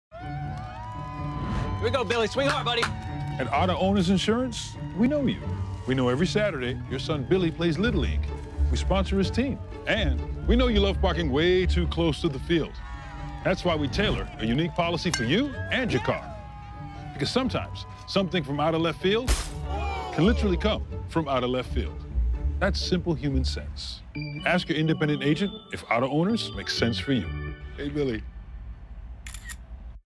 Car Insurance Commercial | Windshield Crack | Auto-Owners
Sometimes life literally comes out of left field. We've got you covered. That's simple human sense. Find out if Auto-Owners Insurance makes sense for you